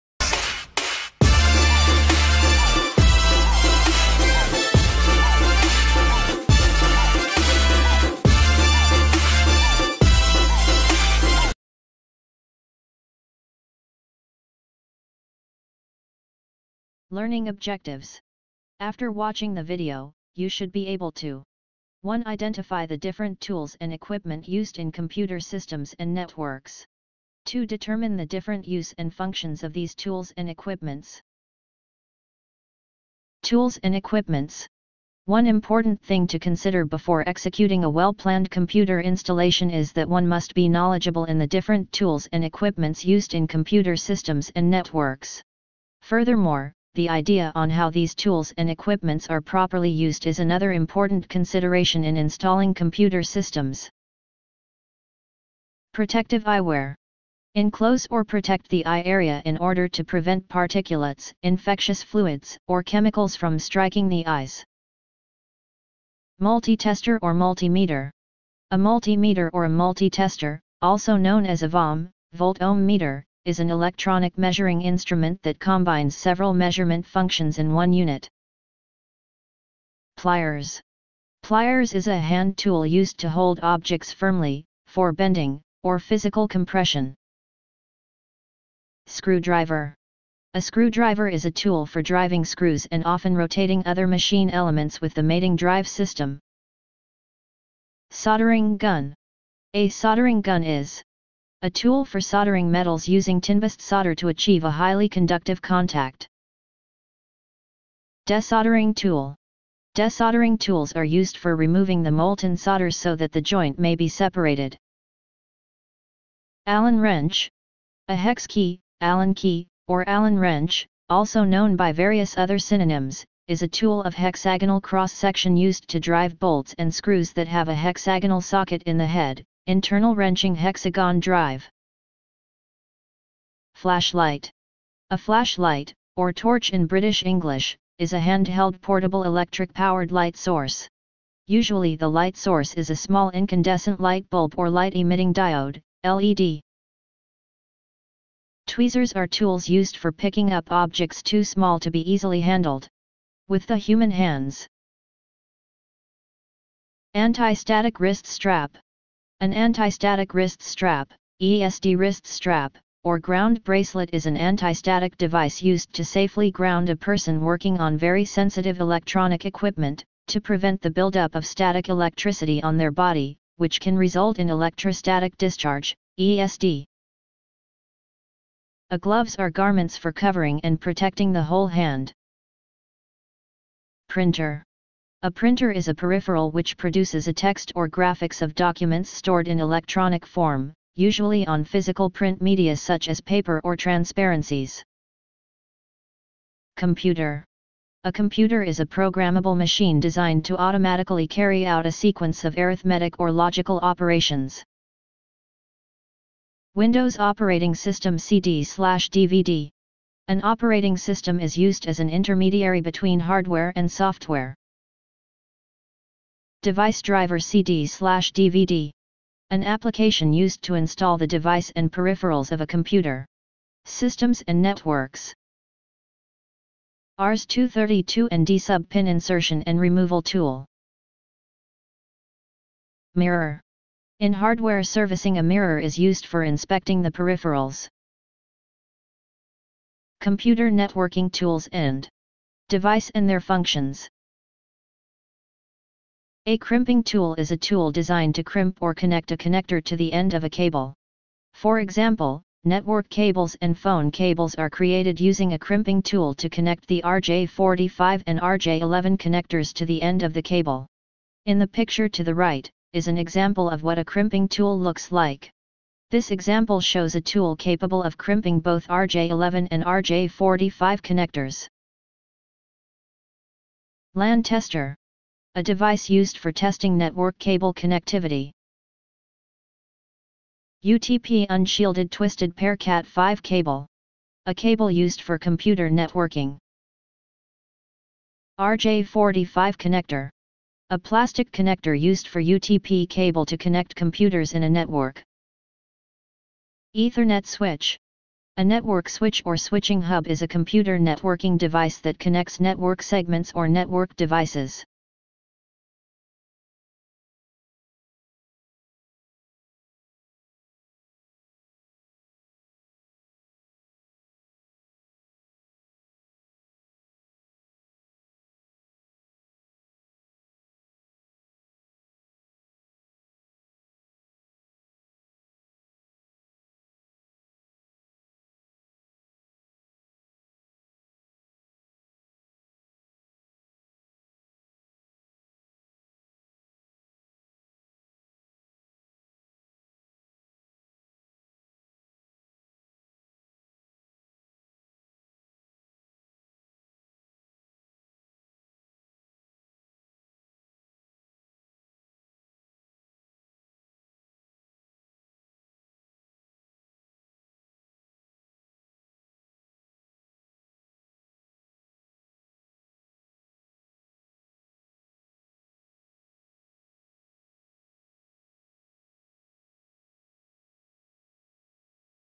TOOLS AND EQUIPMENT|COMPUTER SYSTEMS SERVICING NCII
TOOLS AND EQUIPMENTS

One important thing to consider before executing a well-planned computer installation is that one must be knowledgeable in the different tools and equipment’s used in computer systems and networks. Furthermore, the idea on how these tools and equipment’s are properly used is another important consideration in installing computer systems.